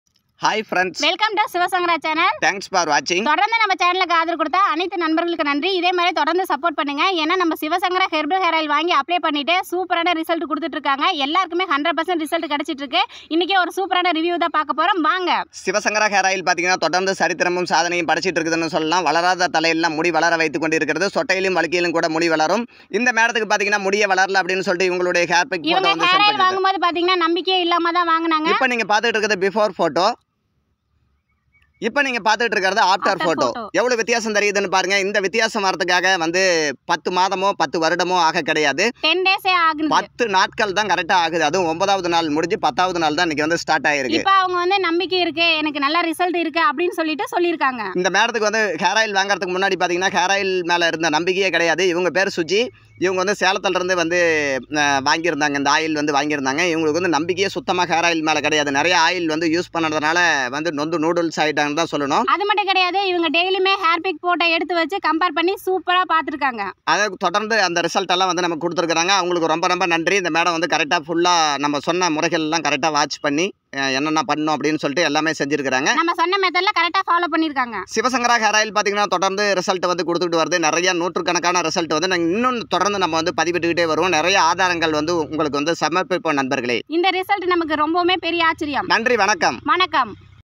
hair growth far fast growth hairoil review/10days result...
Hello friends, in today's post, our hair oil is very good for hair growth, the person who bought our fast growth hair oil has given proof. We are continuously creating many achievements. Those who have benefited from using the oil praise it